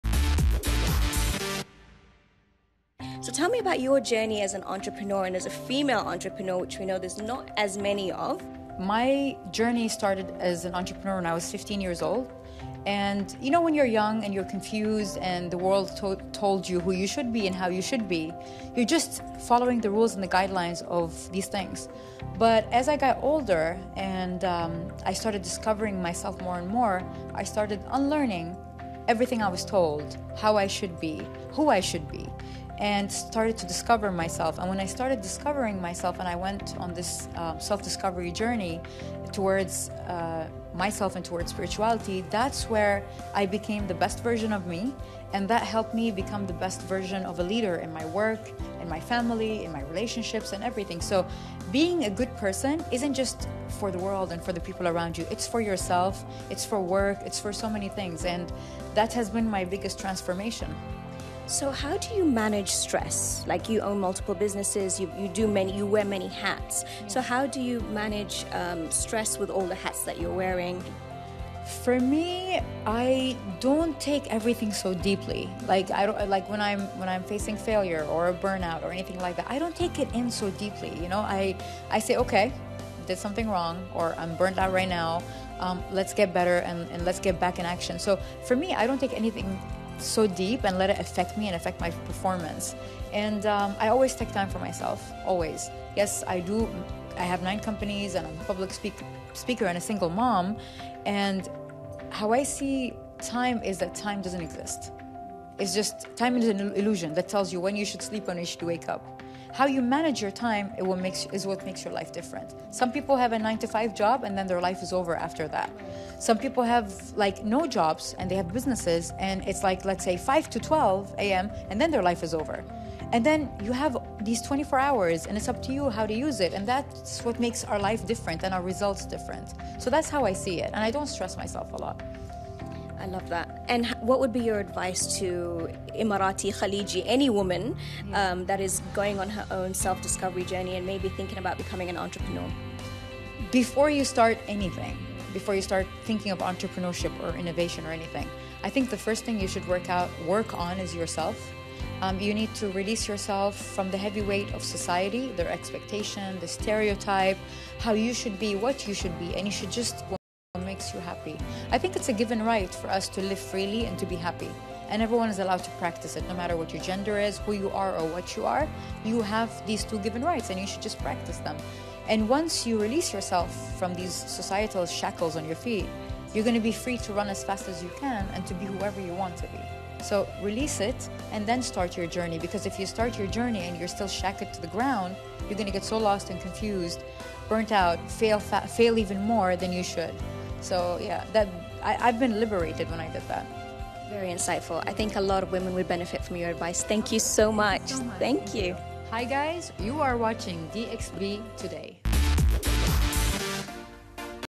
DXB Today Exclusives | Sara Al Madani encourages women to live freely and be happy
Well-known female entrepreneur Sara Al Madani encourages women to live freely and be happy